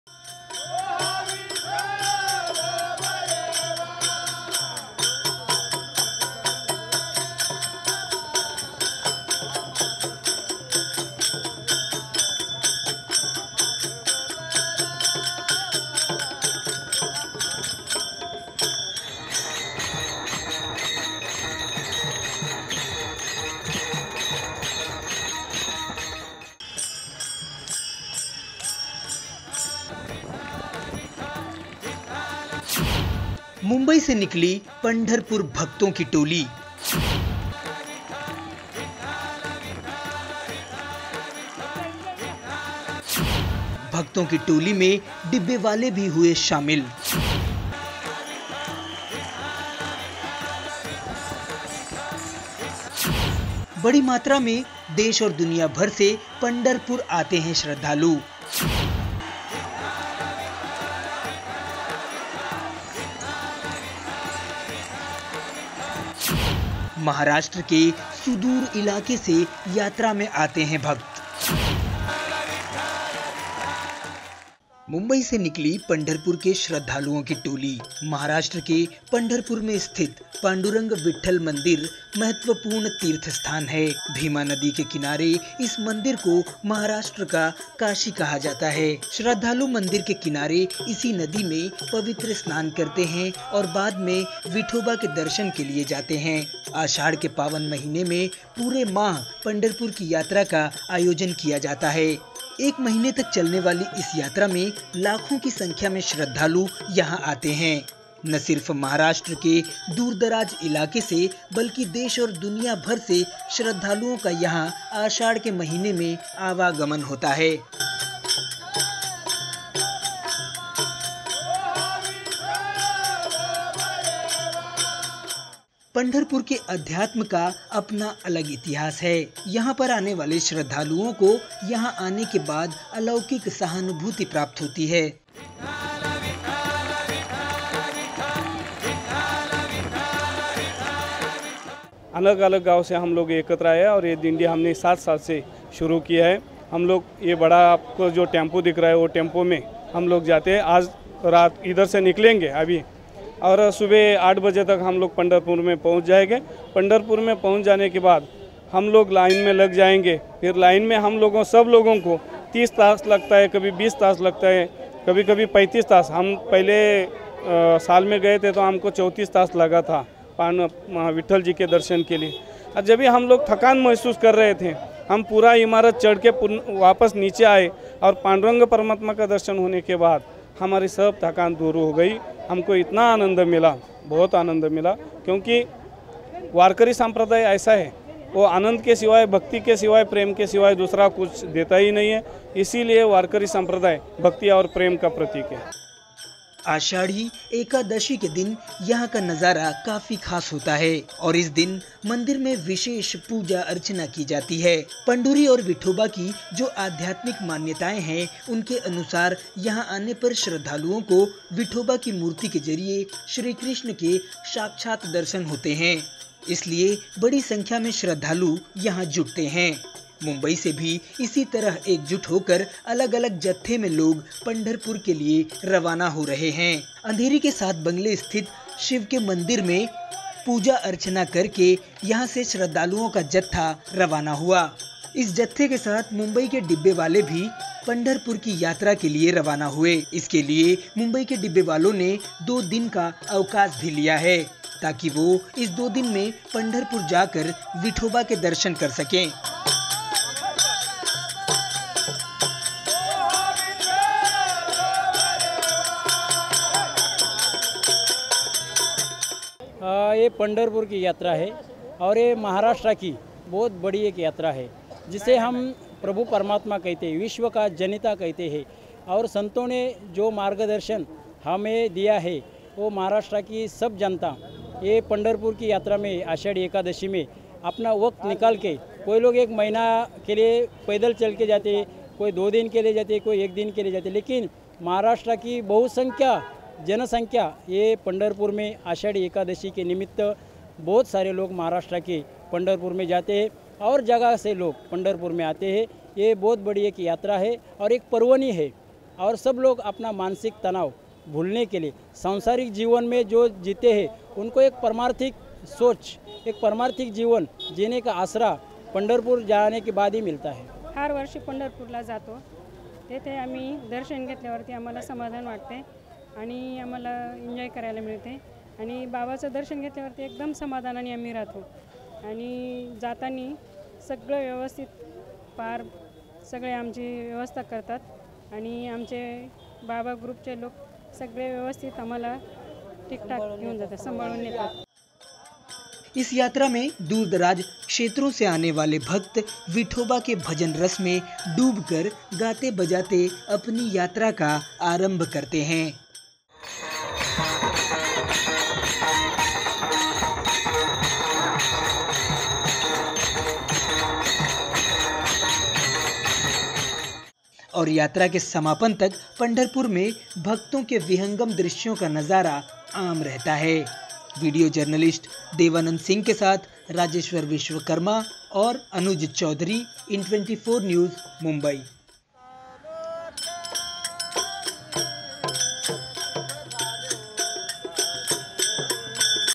Dabbevale Chale Pandharpur in24news
मुंबई से निकली पंढरपुर भक्तों की टोली
भक्तों की टोली में डिब्बेवाले भी हुए शामिल
बड़ी मात्रा में देश और दुनियभर से पंढरपुर आते हैं श्रद्धालु
महाराष्ट्र के सुदूर इलाके से यात्रा में आते हैं भक्त
आषाढ़ के महीने में सदियों से यात्रा की परंपरा कायम
आषाढ़ी एकादशी को होता है पंढरपुर में खास आयोज़ंन